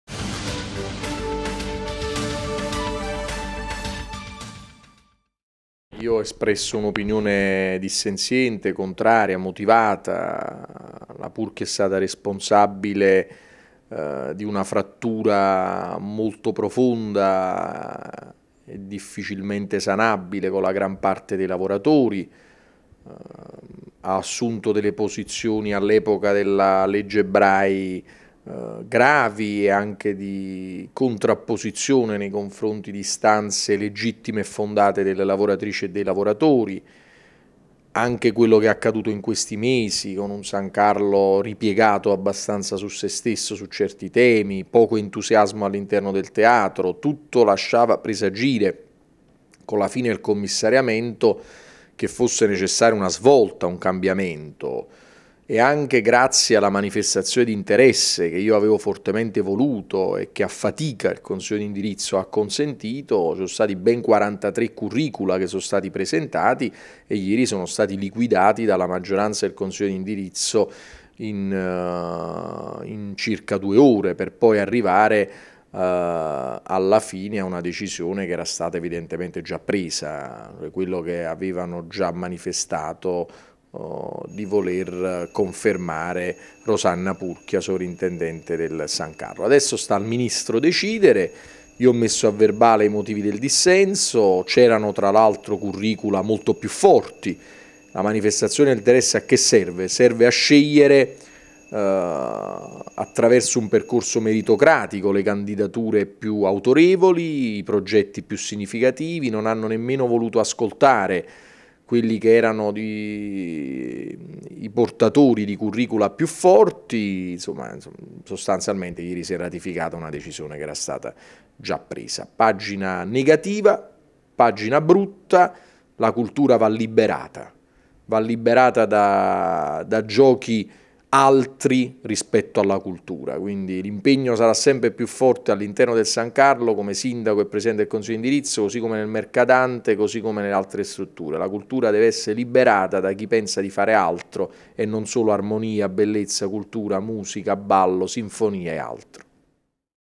Sindaco: «Per il San Carlo quarantatré candidature liquidate in due ore»
Il sindaco de Magistris commenta la decisione del consiglio di indirizzo del Teatro San Carlo, che fra le varie candidature pervenute ha scelto di riconfermare Rosanna Purchia come soprintendente, col solo voto contrario dello stesso de Magistris.